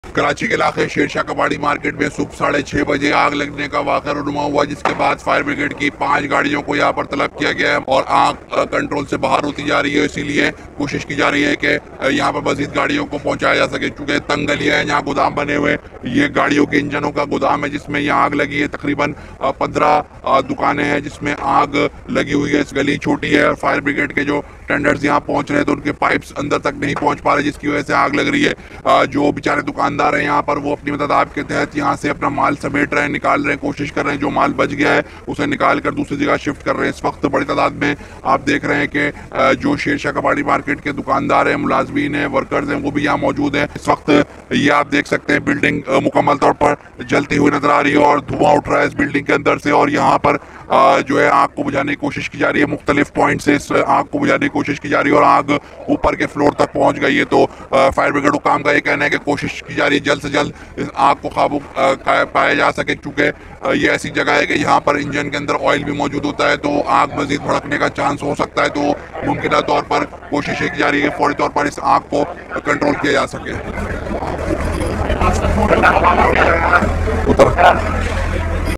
Fire breaks out in Karachi’s Shershah Kabari Market | Press TV
Breaking News...

پریس ٹی وی کے مطابق، جمعرات کی صبح کراچی کی شیرشاہ کباری مارکیٹ میں کئی دکانوں میں آگ لگ گئی۔

آگ نے کچھ ہی دیر میں مارکیٹ کی مختلف دکانوں کو اپنی لپیٹ میں لے لیا۔ فائر بریگیڈ کی کم از کم 10 گاڑیاں موقع پر پہنچ گئیں اور آگ بجھانے کے آپریشن میں حصہ لے رہی ہیں۔

ڈیوٹی پر موجود گارڈ کے مطابق آگ صبح ساڑھے 6 بجے کے قریب ایک دکان میں لگی جس نے شارٹ سرکٹ کے باعث ملحقہ کئی دکانوں کو لپیٹ میں لے لیا۔

ریسکیو حکام کا کہنا ہے کہ واقعے میں کوئی جانی نقصان نہیں ہوا۔

تاہم لاکھوں روپے مالیت کا قیمتی سامان راکھ ہو گیا۔